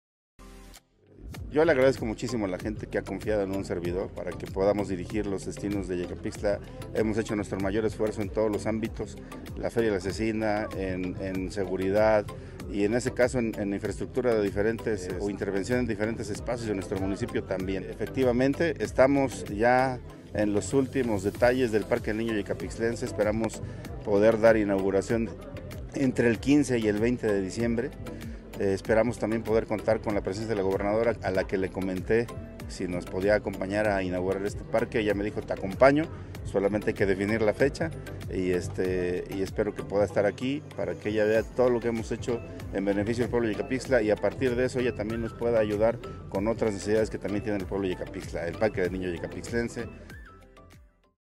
AVANCES EN EL PARQUE DE LA NIÑEZ YECAPIXTLENSE.
En entrevista con Heladio Rafael Sánchez Zavala, presidente municipal de Yecapixtla, informó que el Parque de la Niñez Yecapixtlense está en los últimos detalles previó a su inauguración.